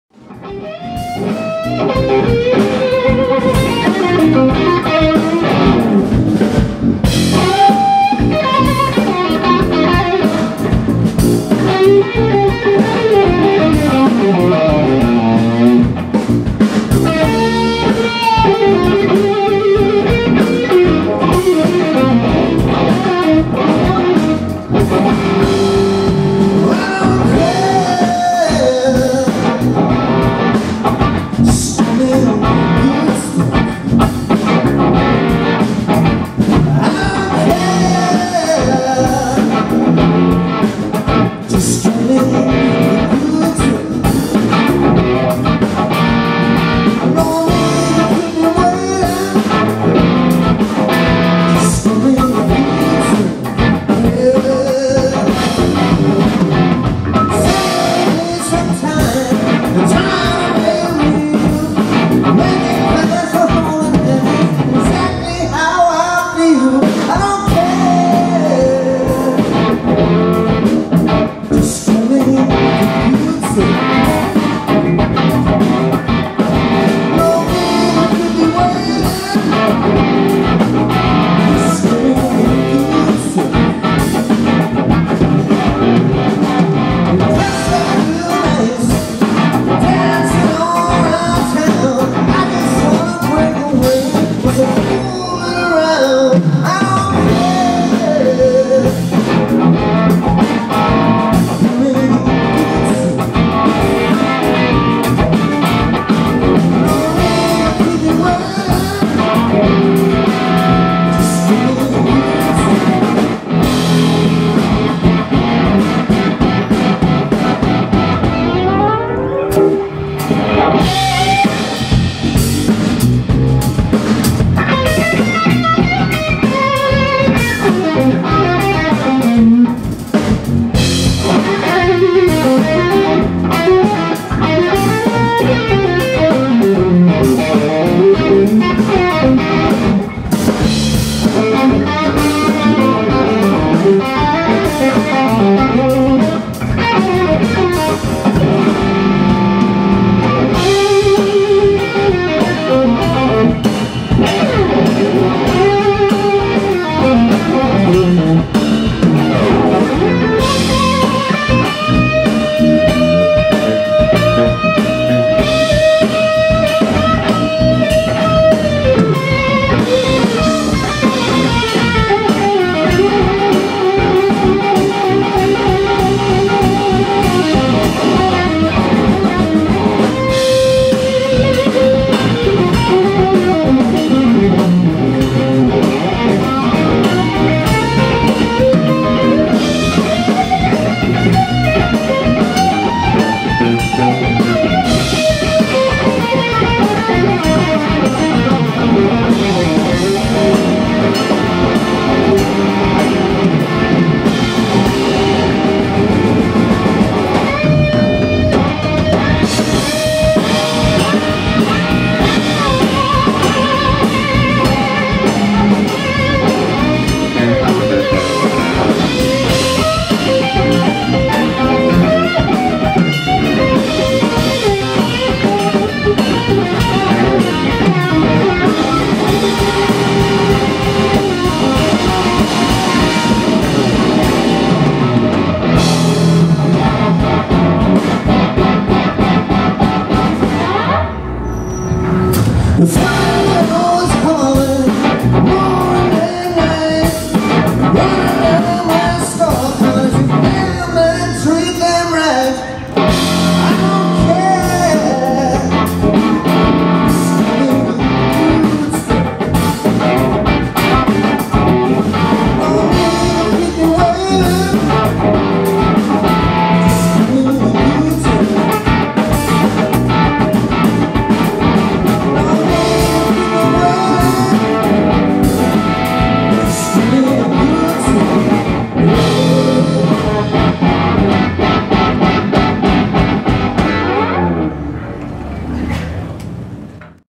CRF 2018 - The Mark Pontin Group
Cambridge Rock Festival July 28th 2018. The Mark Pontin Group performing on the Ruby stage. Filmed by Andy Kitson.